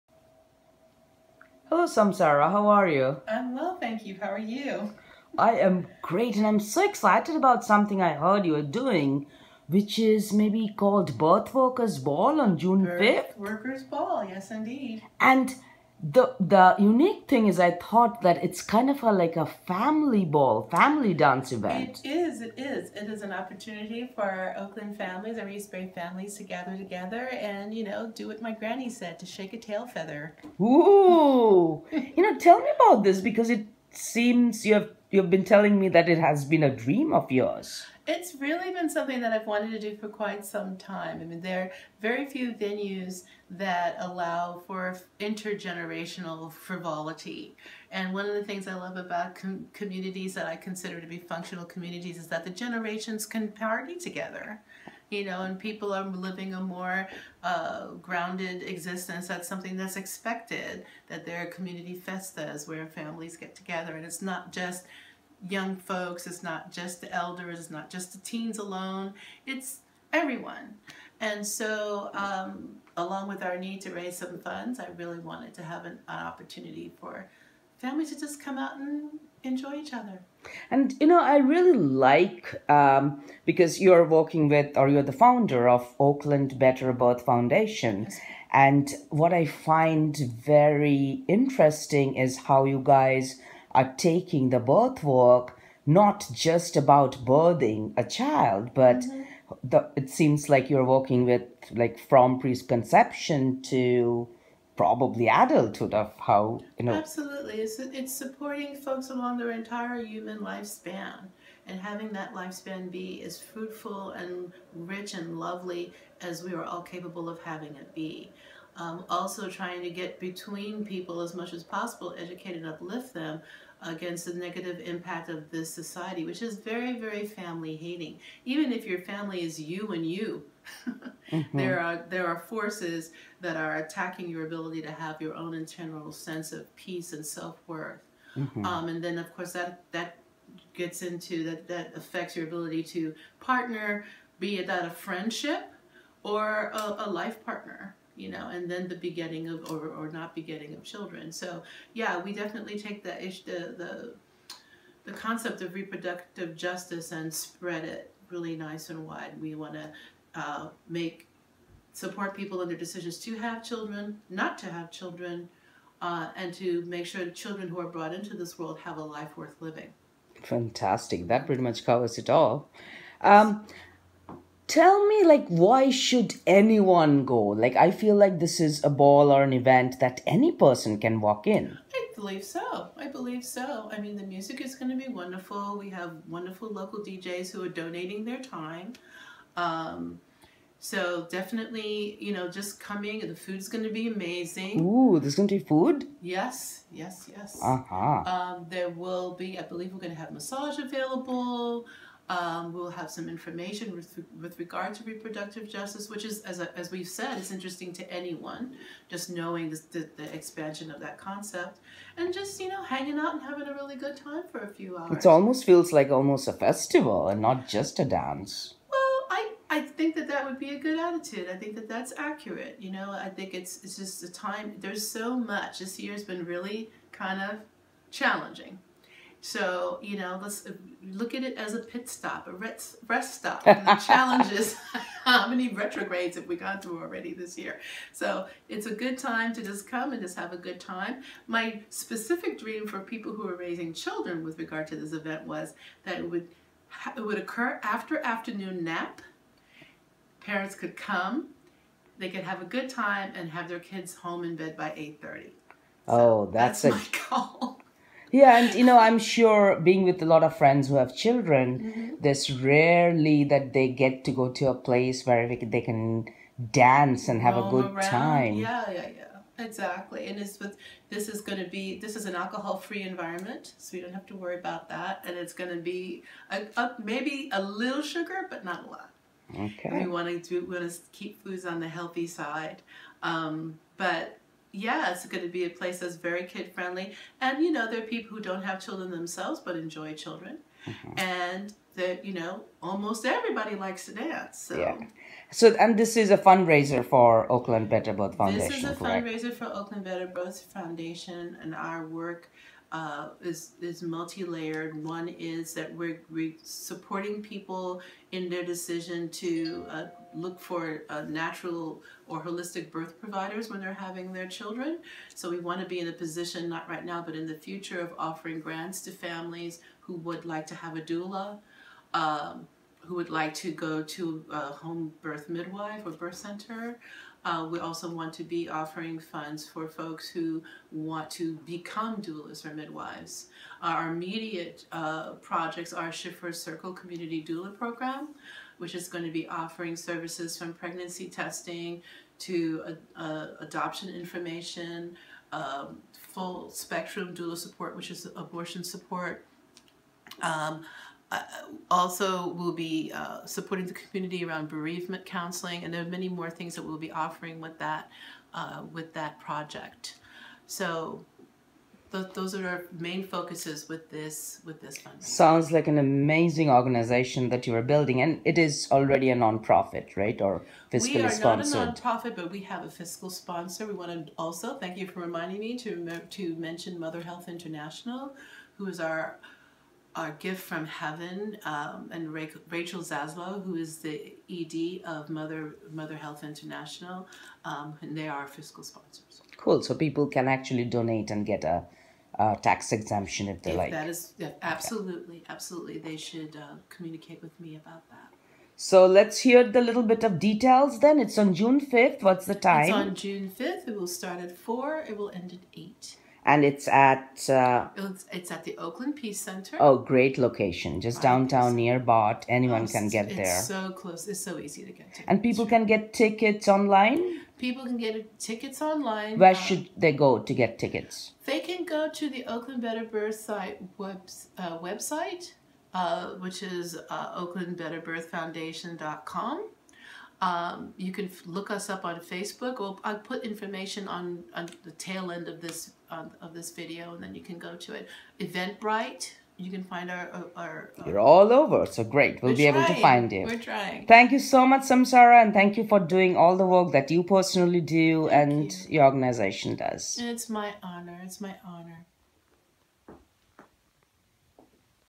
Birthworkers Ball Roke Noir interviews Samsarah Morgan
Join us for the Birthworkers Ball a Family Dance and Fundraiser for the Oakland Better Birth Foundation.
 Tickets are also for sale at the door and no one will be turned away due to finances.
Come dance with us !